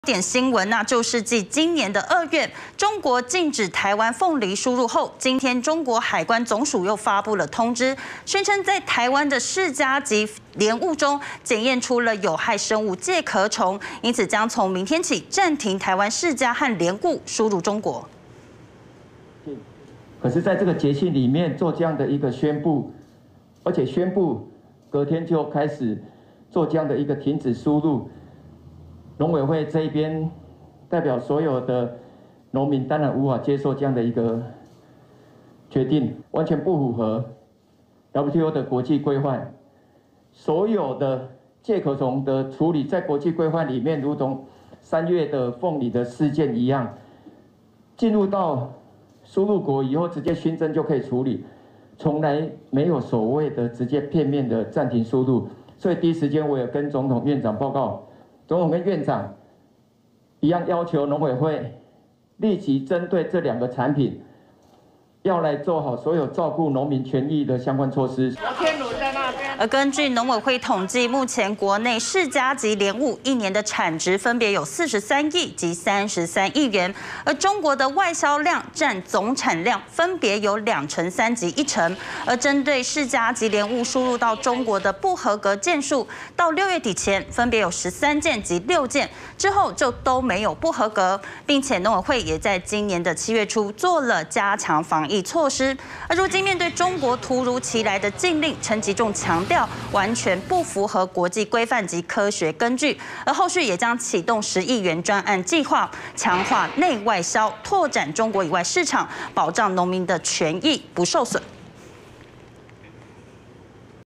再禁台灣農產 明起釋迦.蓮霧暫別中國市場 2021-09-19 IPCF-TITV 原文會 原視新聞
來看今天的焦點新聞，那就是繼今年二月，中國禁止台灣鳳梨輸入後，今天(19)中國海關總署發布通知，宣稱在台灣釋迦及蓮霧中，檢驗出"有害生物"介殼蟲，因此將從明天起(20)暫停台灣釋迦和蓮霧，輸入中國。

根據農委會統計，目前國內釋迦及蓮霧一年產值分別有43億及33億元，中國外銷量佔總產量分別有2成3及1成，而針對釋迦及蓮霧輸入到中國的不合格件數，到六月底前，分別有13件及6件，之後就都沒有不合格，並且農委會也在今年7月初做了加強防疫措施，如今面對中國突如其來的禁令，陳吉仲強調，完全不符合國際規範及科學根據，後續也將啟動十億元專案計畫，強化內外銷，拓展中國以外市場，保障農民權益不受損。